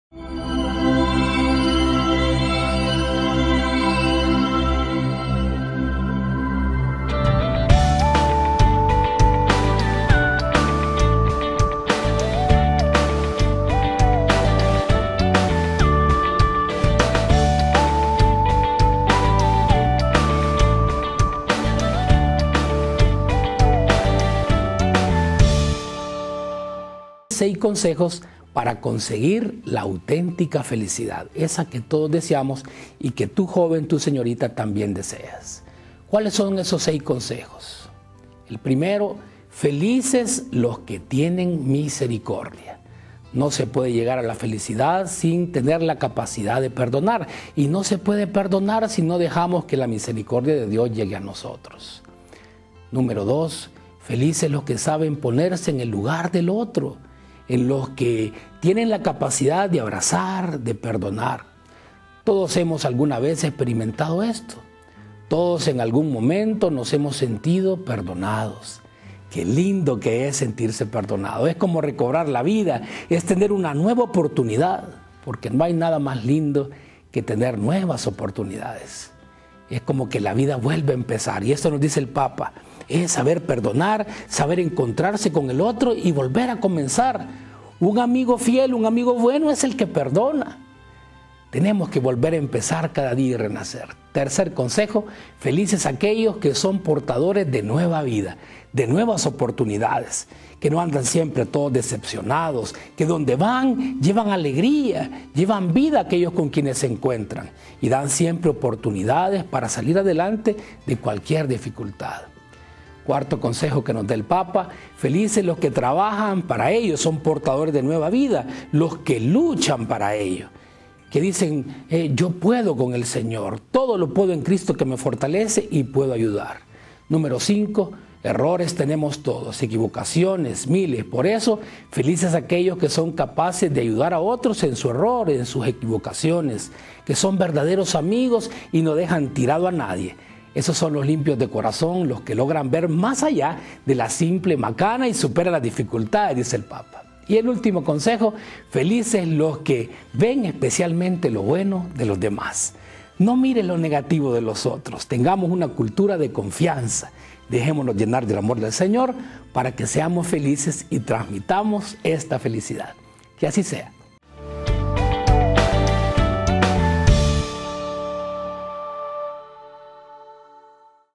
Minuto de María - Consejos para conseguir la autentica felicidad
Padre César Sánchez nos comparte 6 consejos para conseguir la autentica felicidad.

Dale play a nuestra cápsula diaria